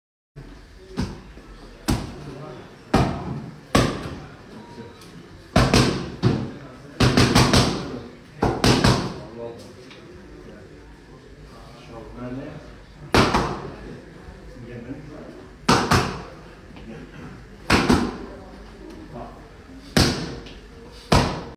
JOHN RYDER ON THE PADS MOMENTS BEFORE CANELO FIGHT! 🥊
(Video courtesy of Matchroom)